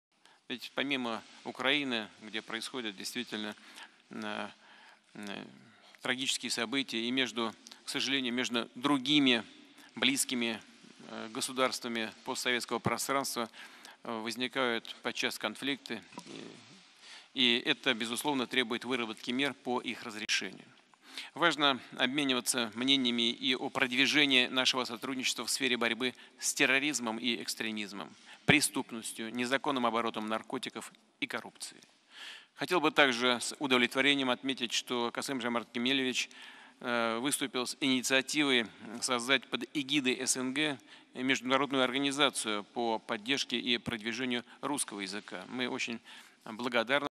⚡️Vladimir Putin spoke at an informal summit of the heads of the CIS in St. Petersburg.
Main statements:

✅ The Russian President will take part in the first-ever Russia-Central Asia summit in Astana, where priority will be given to the integration of the Commonwealth countries in politics, economy, other areas, as well as security issues;

✅ The tragic events in Ukraine and conflicts between other CIS countries require the development of measures to resolve them, in the context of which it is extremely important to continue cooperation in the fight against terrorism and extremism;

✅ The Russian language is the language of interstate communication, in connection with which Russia is grateful to President Tokayev for his initiative to create an international organization under the auspices of the CIS to support and promote the Russian language;

✅ Last year, Russia's trade with the CIS countries grew by 30% to $96 billion;

✅ In the future, such positive trends will be developed by an energetic transition to national currencies in mutual settlements.

if not then you're an asshole